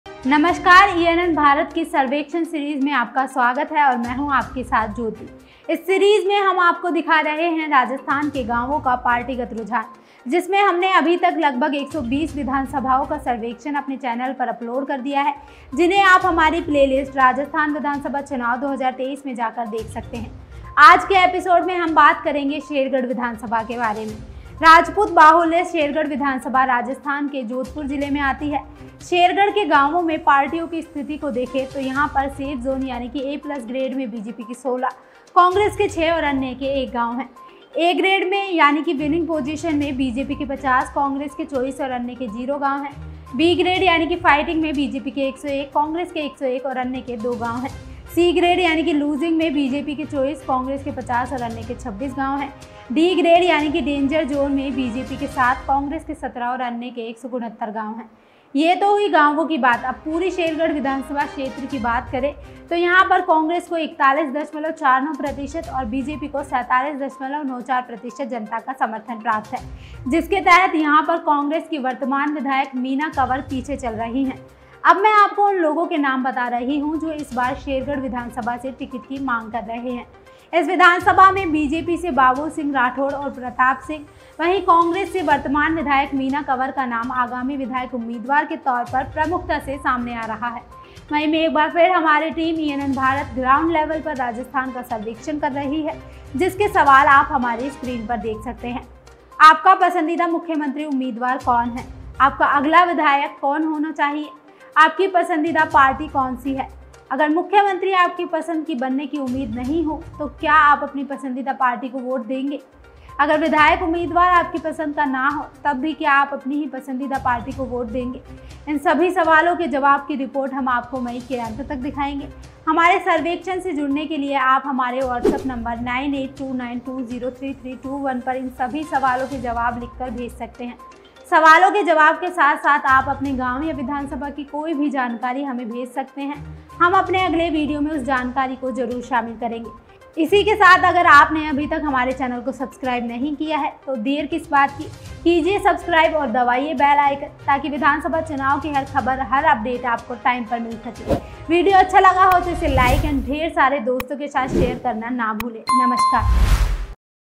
शेरगढ़ विधानसभा चुनाव 2023, सर्वेक्षण || Rajasthan Politics
News Description : इस सीरीज में हम आपको दिखा रहे हैं राजस्थान के गावों का पार्टीगत रुझान। जिसमें हमने लगभग 120 विधानसभाओं सर्वेक्षण अपने चैनल पर अपलोड़ कर दिया है। जिन्हें आप हमारी प्लेलिस्ट राजस्थान विधानसभा चुनाव 2023, सर्वेक्षण में जाकर देख सकते है। आज के एपिसोड में हम बात करेंगें शेरगढ़ विधानसभा के बारे में।